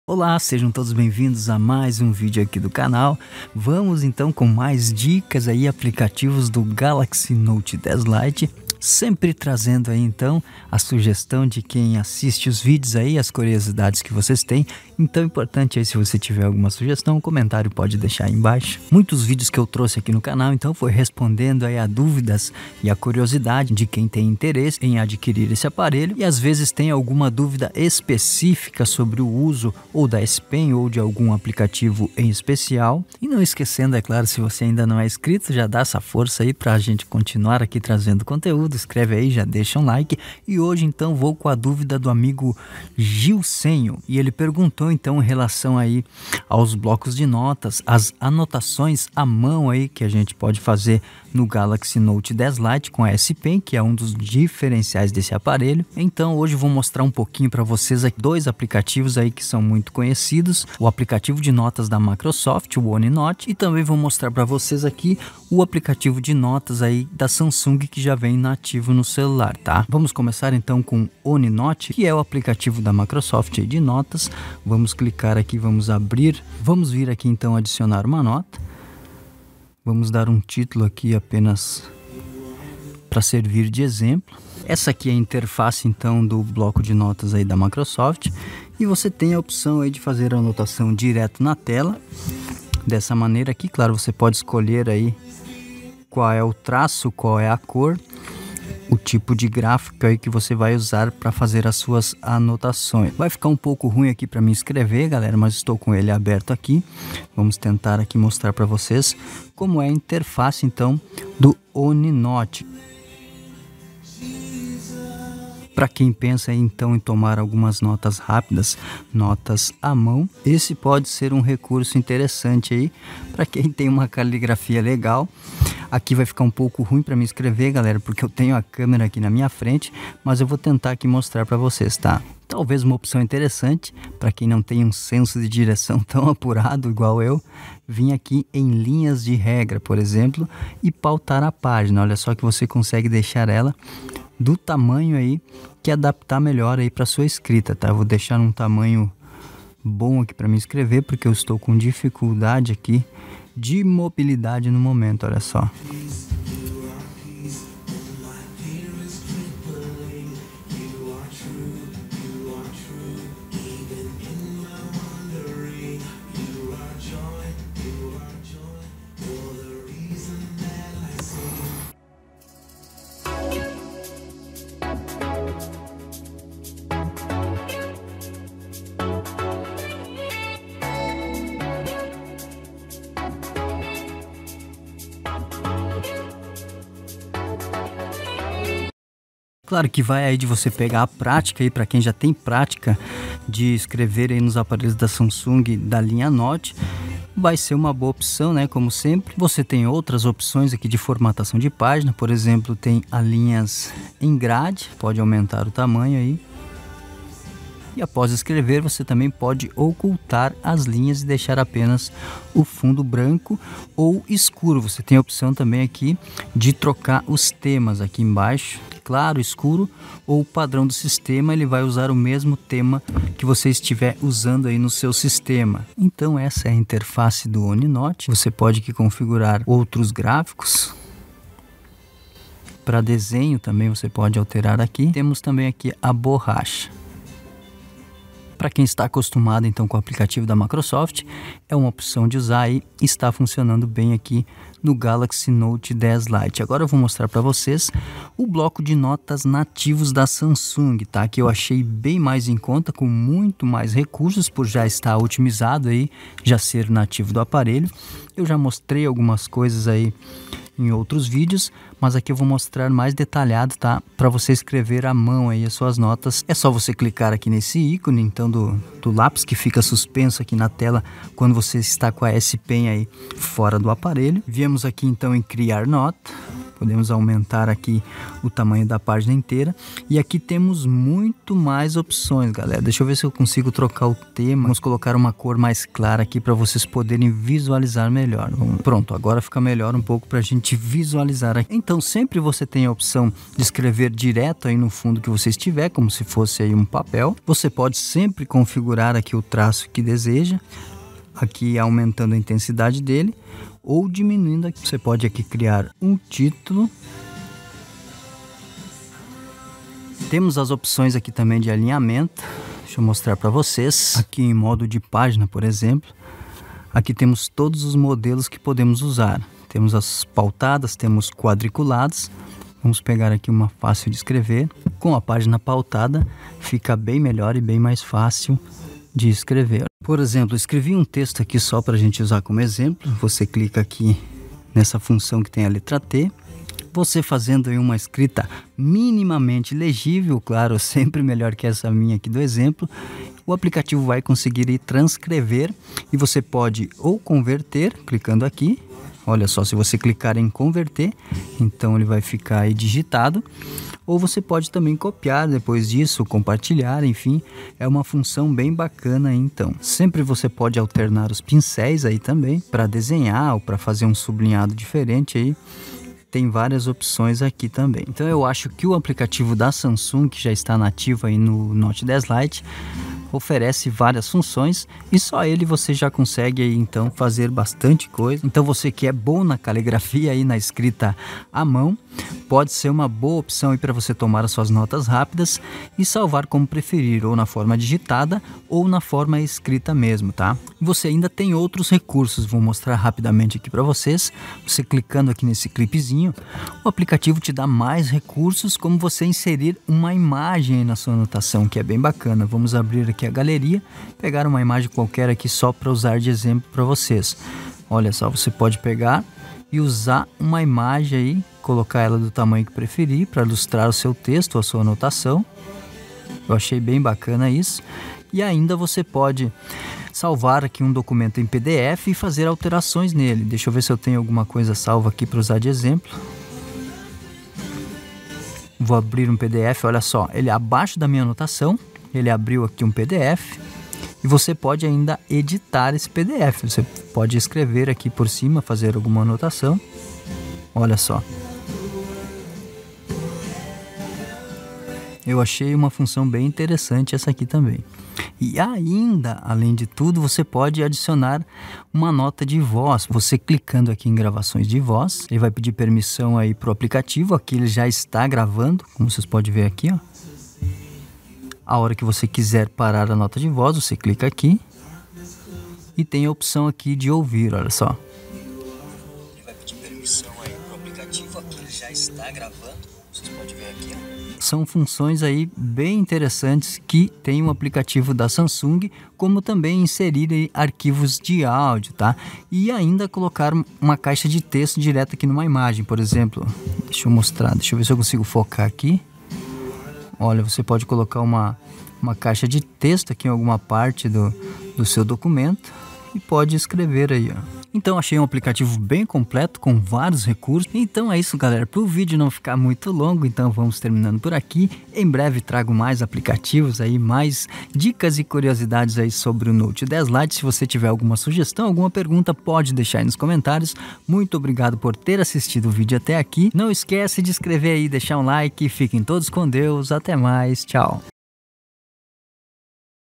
GALAXY NOTE 10 LITE DICAS DE APPS PARA ANOTAÇÕES
FALA GALERA! HOJE VAMOS ANALISAR OS APPS DE BLOCO DE NOTAS PARA ESCRITA A MÃO USANDO A S PEN NO SAMSUNG GALAXY NOTE LITE.
VEJA AS PRINCIPAIS FUNÇÕES DESSES APLICATIVOS PARA ESCRITA COM A S PEN DO NOTE 10 LITE.
DICAS PARA USAR NO SEU DIA A DIA E TOMAR NOTAS RÁPIDAS E ATÉ MESMO USAR NOS SEUS ESTUDOS PARA COLHER DADOS E INFORMAÇÕES.